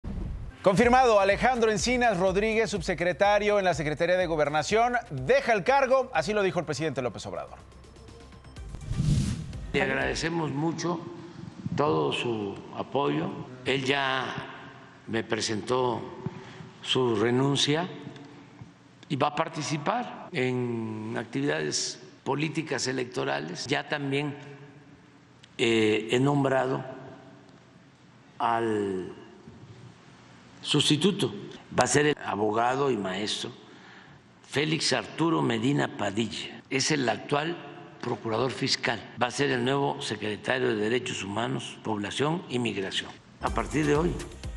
Alejandro Encinas deja su cargo en la SEGOB | DPC con Nacho Lozano
AlejandroEncinas Rodríguez, subsecretario en la Secretaría de Gobernación, deja el cargo, así lo informó el presidente LópezObrador durante la conferencia matutina.